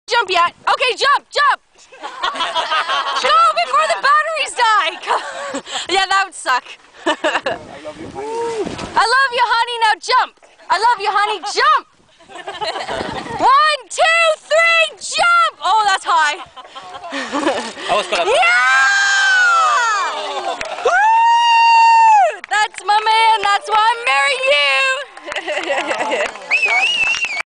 Spinnaker Flying
on the Trimiran Lupita in Mexico 2009